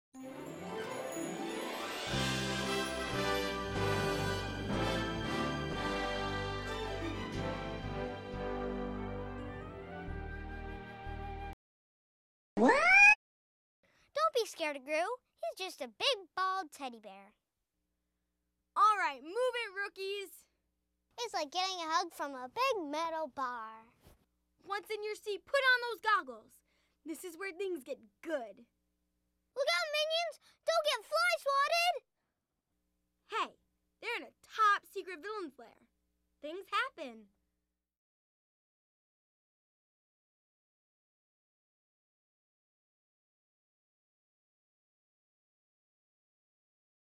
Despicable Me Minion Mayhem at Universal Orlando
Despicable Me Minion Mayhem, will take guests on an unforgettable 3-D adventure with their favorite characters from the hit film.